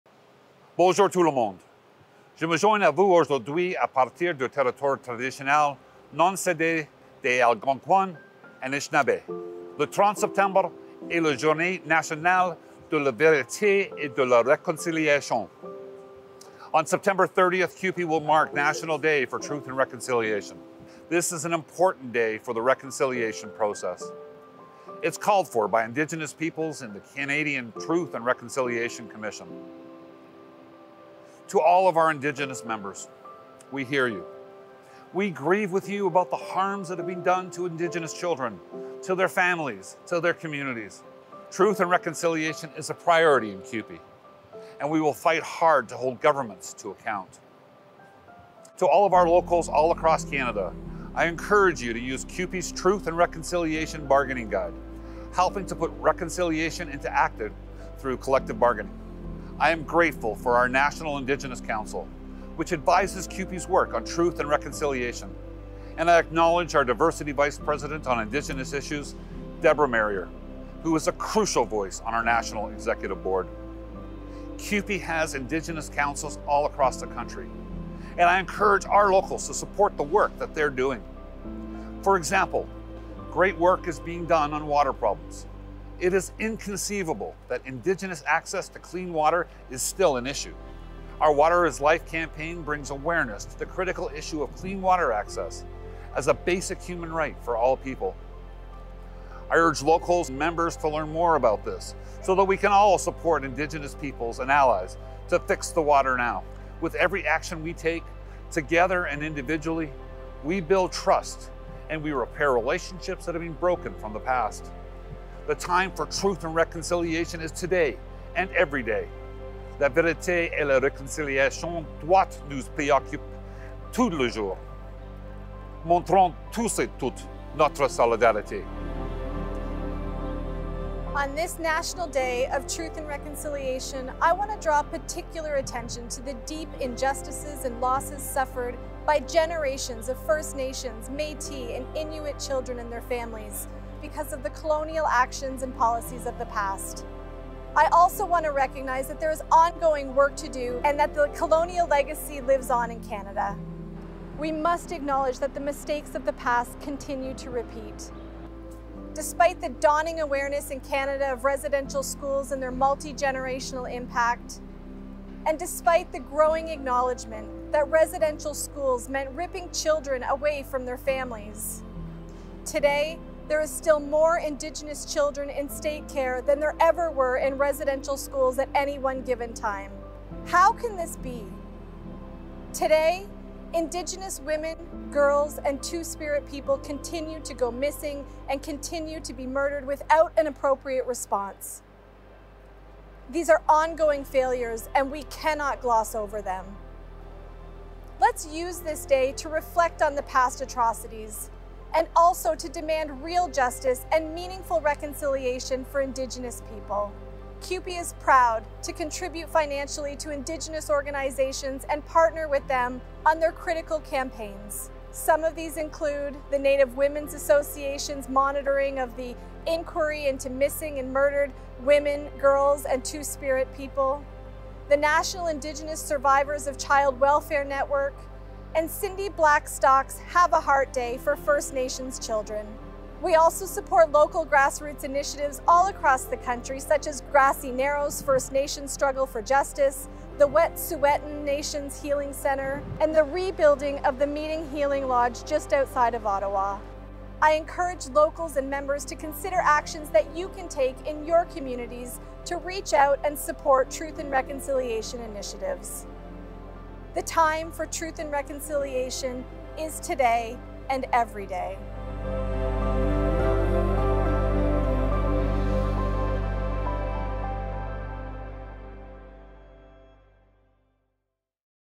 Le SCFP souligne la Journée nationale de la vérité et de la réconciliation
Le 30 septembre 2022, le SCFP souligne la Journée nationale de la vérité et de la réconciliation. Cette journée est un élément important du processus de réconciliation. Elle a été réclamée par les peuples autochtones et la Commission de vérité et réconciliation du Canada.

La vérité et la réconciliation sont une priorité au SCFP. Le président national du SCFP, Mark Hancock, et la secrétaire-trésorière nationale du SCFP, Candace Rennick, soulignent notre engagement à continuer de nous battre pour obliger les gouvernements à rendre des comptes.

Le SCFP a des conseils autochtones partout au pays. Nos sections locales sont encouragées à appuyer leur travail. Elles ont aussi accès au Guide pour la négociation favorisant la vérité et la réconciliation du SCFP, qui les aide à avancer vers la réconciliation par la négociation collective.

Chaque action que nous entreprenons, ensemble et individuellement, aide à bâtir des relations de confiance et à réparer nos liens rompus par le passé.

L’heure de la vérité et de la réconciliation, c’est maintenant, et tous les jours.